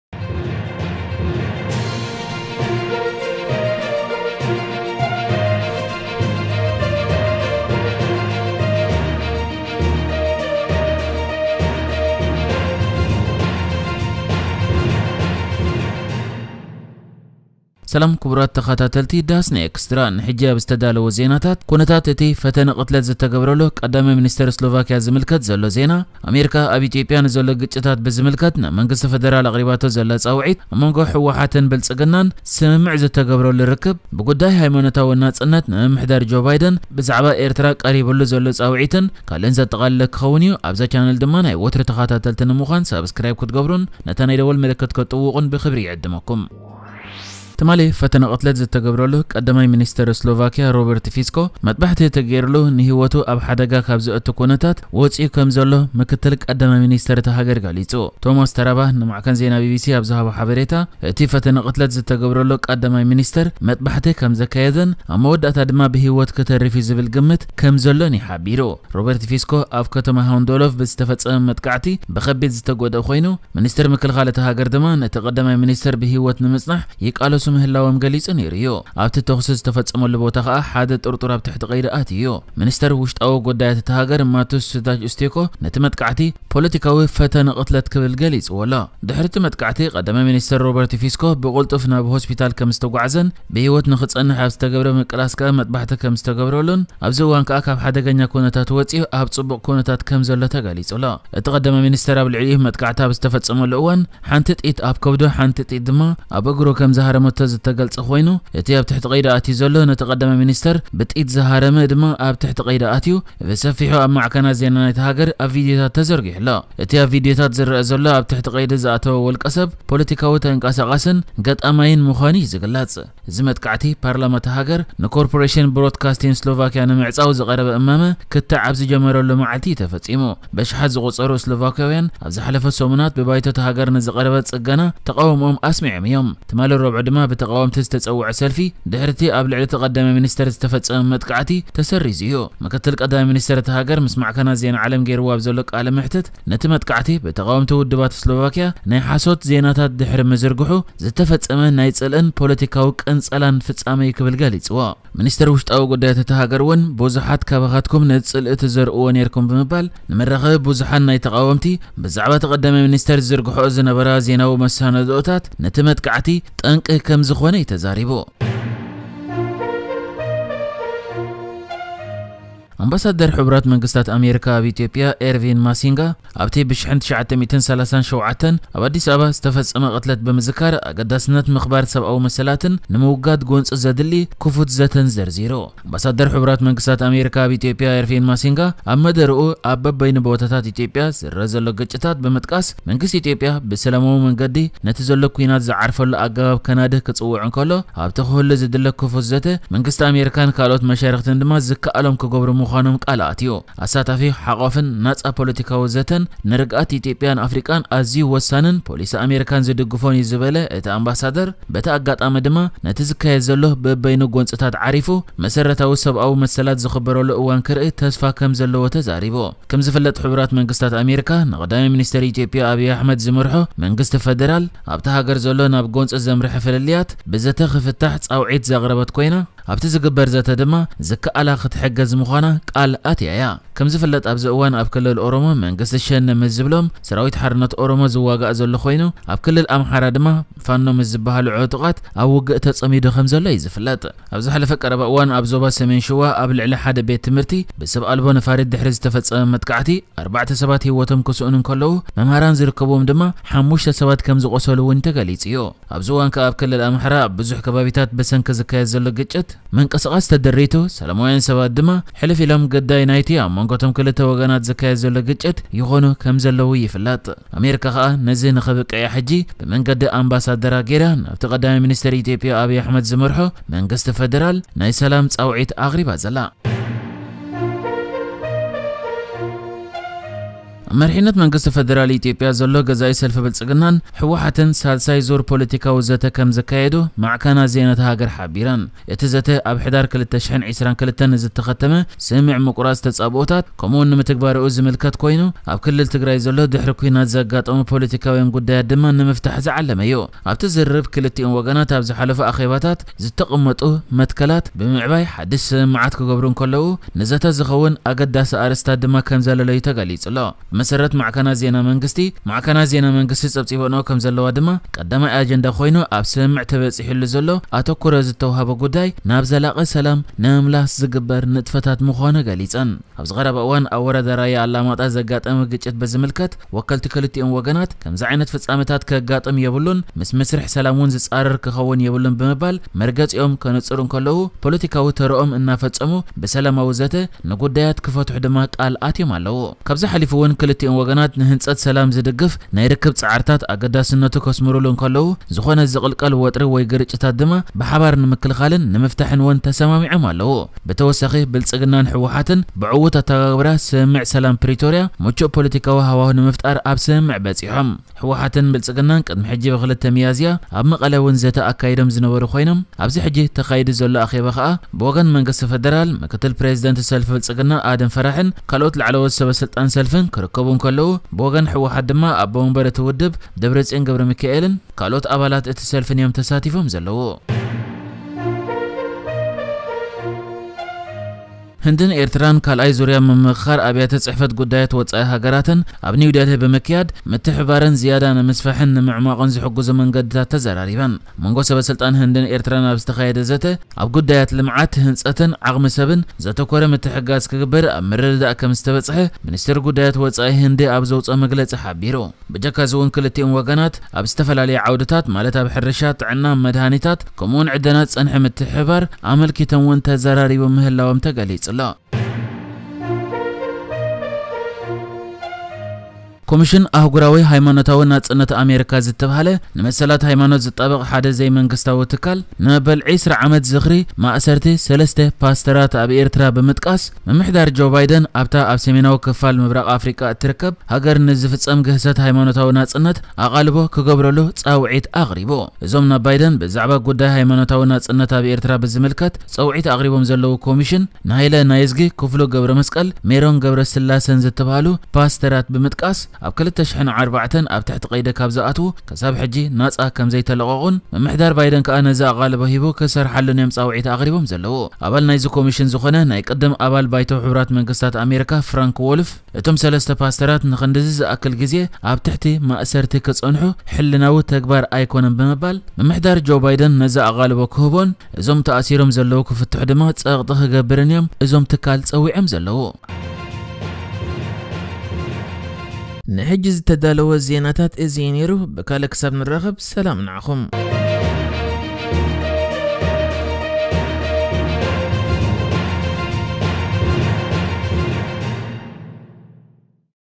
( ሓድሽ ዜናታት ትግርኛ ) - ባይደን ንኤርትራ ዝቀረበሉ - ህወሓት ብልጽግና ስምምዕ - ZENA TIGRIGNA 16/05/2024
© መሰል ናይዛ ቪድዮ ን DasnaExtra ዝተሓዝአ እዩ